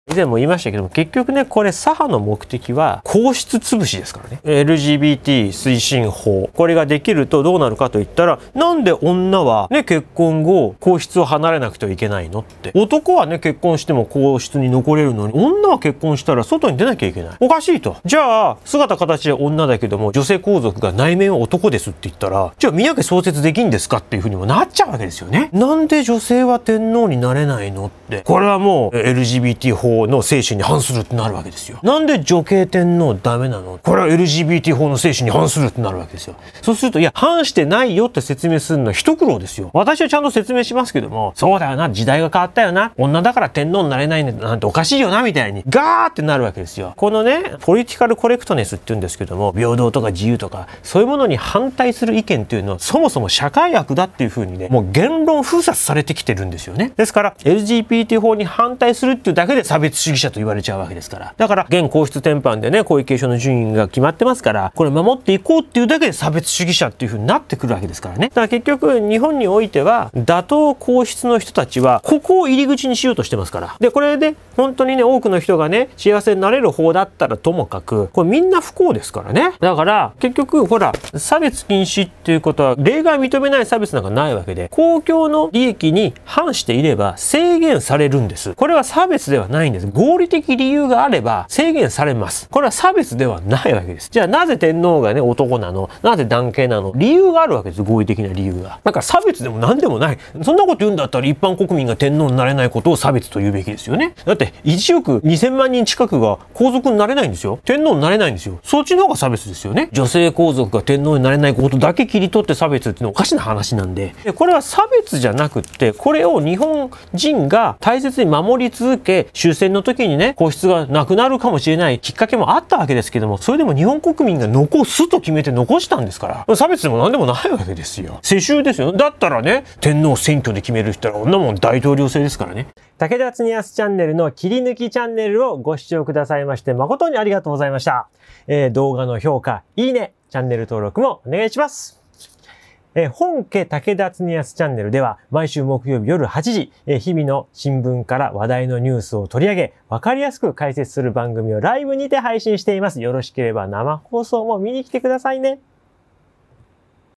LGBT法案は左派の皇室つぶし！
LGBT法案が出来れば将来的に
女性皇族が内面が男性だから宮家創設できる！という話になる！
LGBT法案は皇室潰しにつながります！